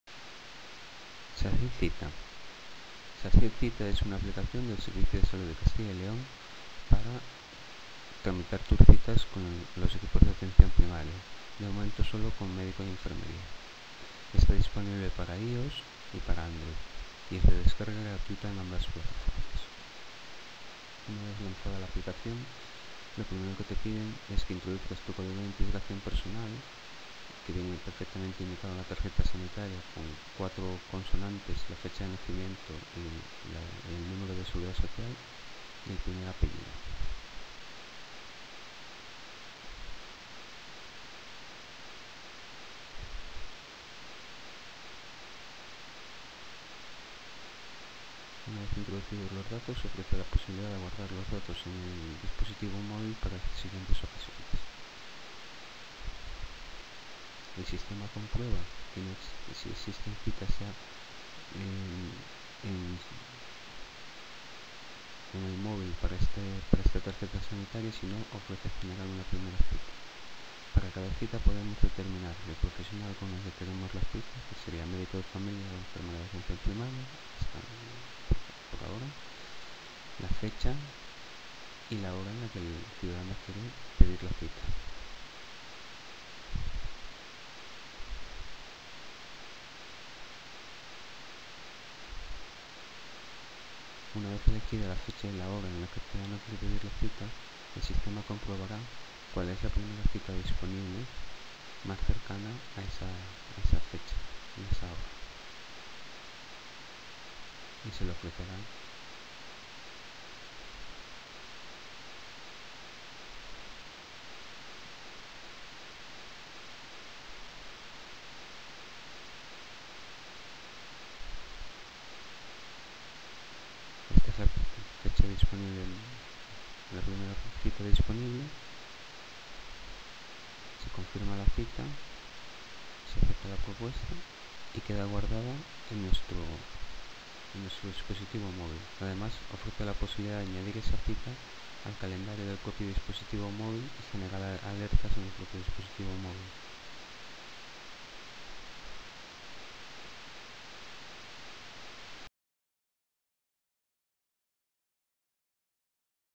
Sacyl_cita
Video captura del uso de la App Sacyl_cita